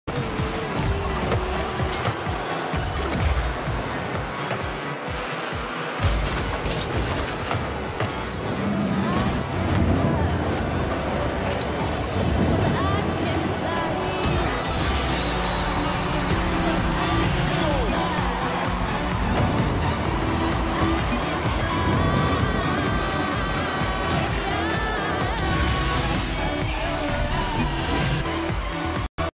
This needs to be on Potato League
Sure, I'm no Grand Champion in Rumble, but how often do you see a shot like THIS???